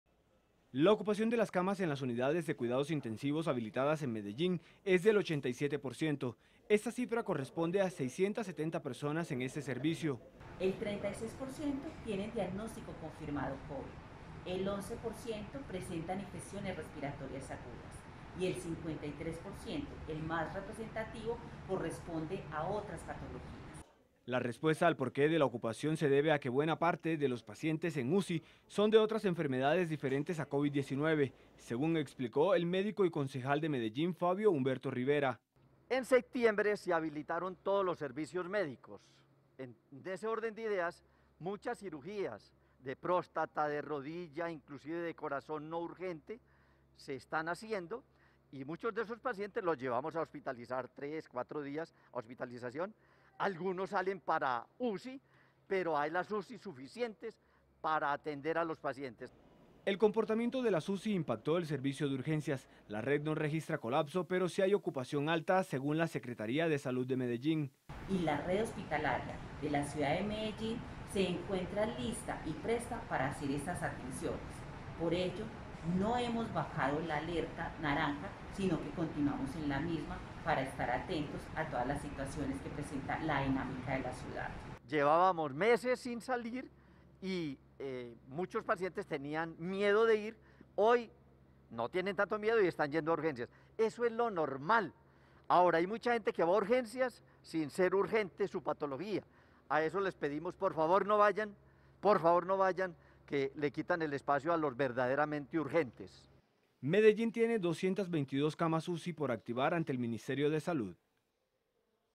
Esta es la situación actual del sistema de salud en Medellín - Telemedellín
La Secretaría de Salud entregó una radiografía de cómo está actualmente el sistema de salud de la ciudad. A la autoridad sanitaria se suman concejales que explican detalles de la ocupación de las Unidades de Cuidados Intensivos y los servicios de urgencias.